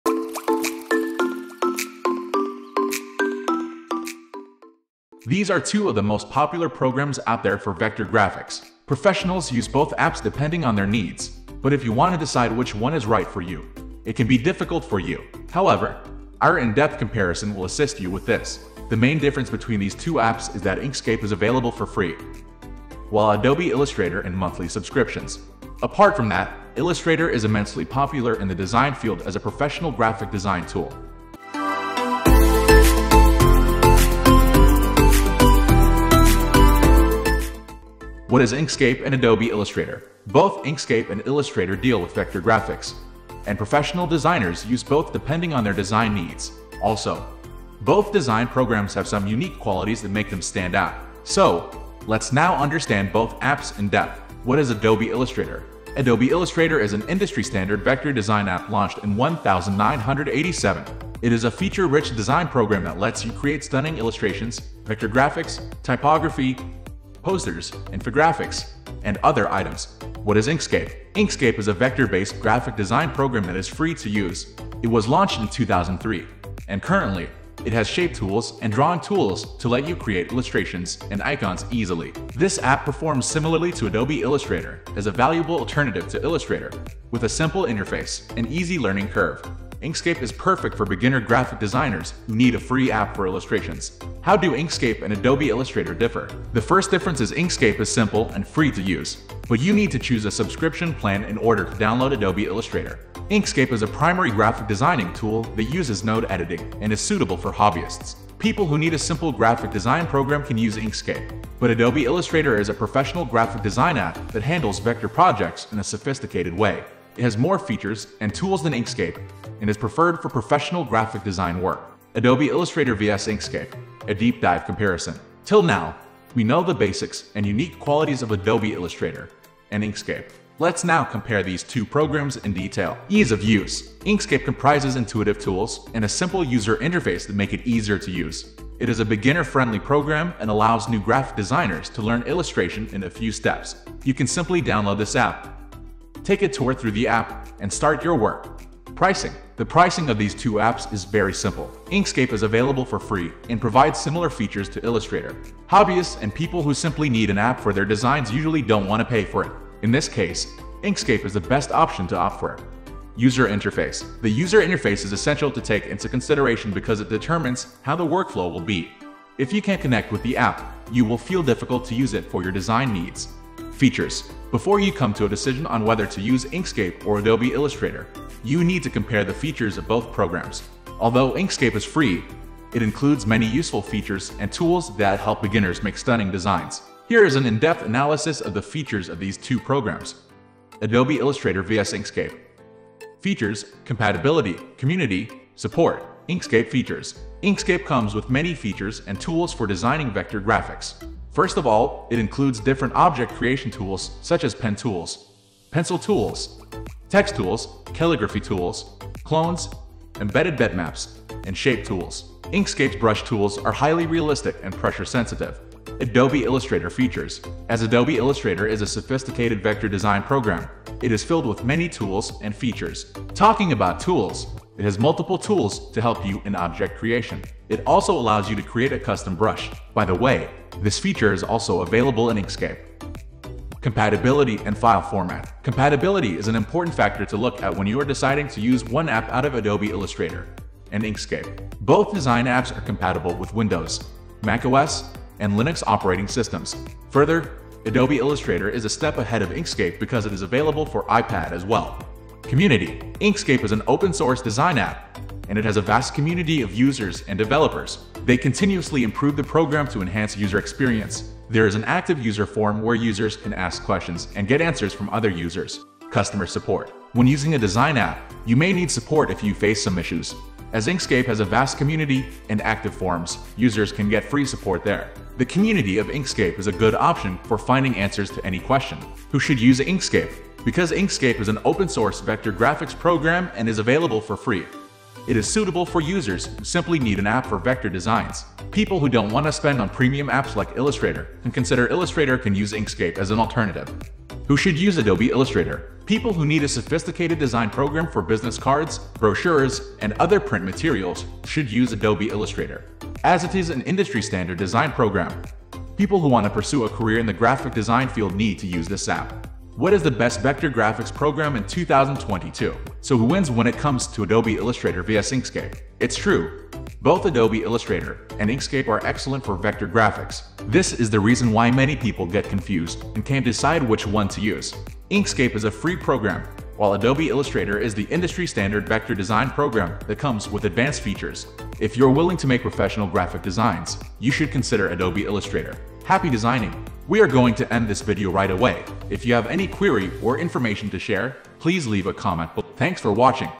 Adobe Illustrator vs Inkscape: A Complete Comparison – What is The Best Vector Graphics Program 2025
These are two of the most popular programs out there for vector graphics. Professionals use both apps depending on their needs. But if you want to decide which one is right for you, it can be difficult for you. However, our in-depth comparison will assist you with this.

Adobe Illustrator | Vector graphic design software

Inkscape - Illustration (Vector Drawing) Software